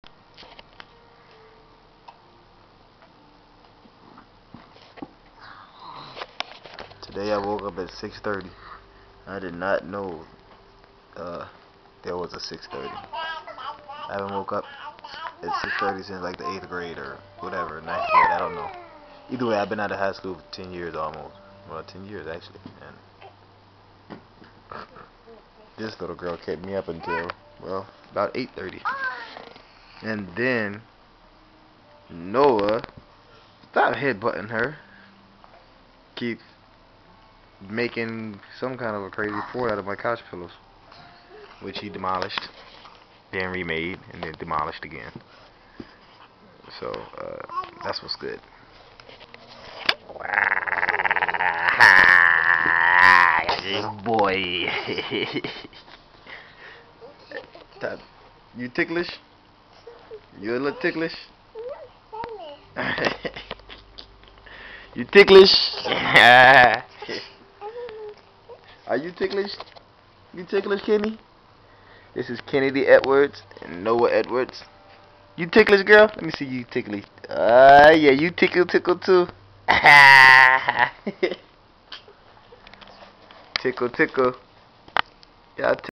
Noah and Kennedy Edwards
This is footage of me watching the Edward kids Thursday (Sept 4th 2008). Love these Kids!